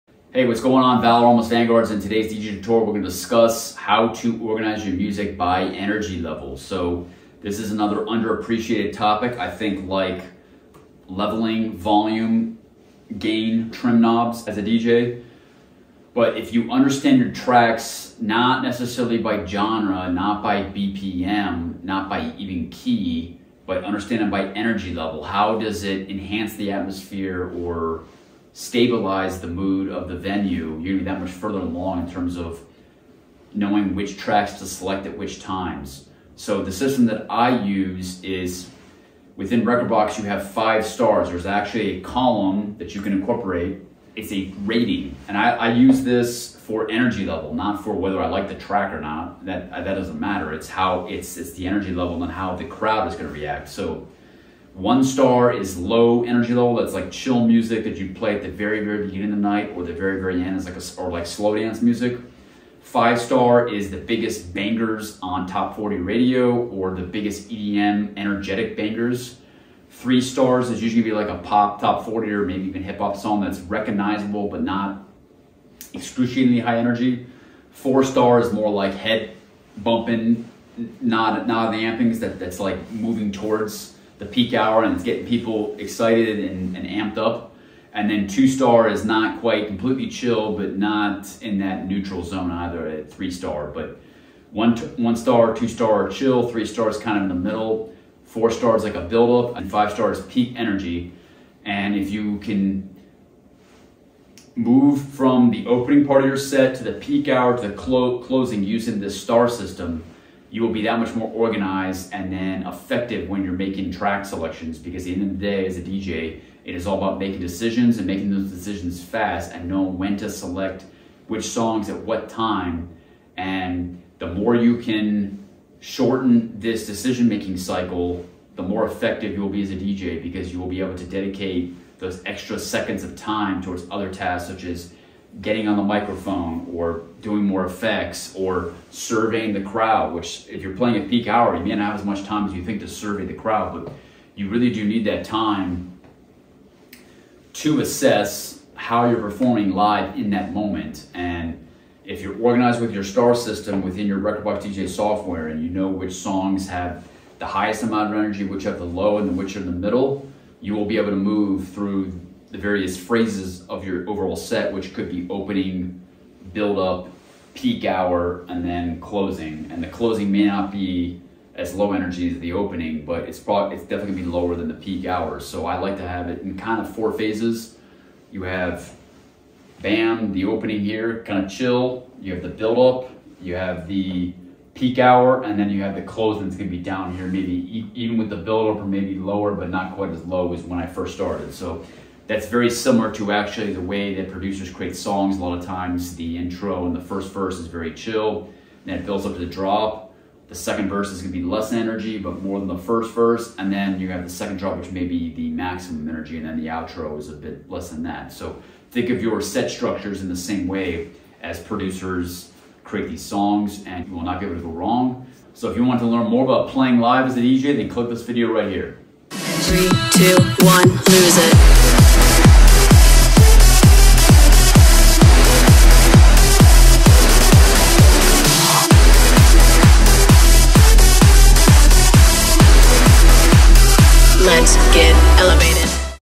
DJ Tutorial 144 | Mark Tracks By Energy Level Within Rekordbox DJ
In this video, I explain my system for rating songs within Rekordbox DJ software, which allows me to manage gigs throughout time.

Valoramous:
-🇺🇸 Jetsetter ✈️
-“The James Bond of Producers” 👔 💼 ⏱ 🧤 🛂
-DJ/Producer 🎧: Big Room, Rave-Rock, Synthpop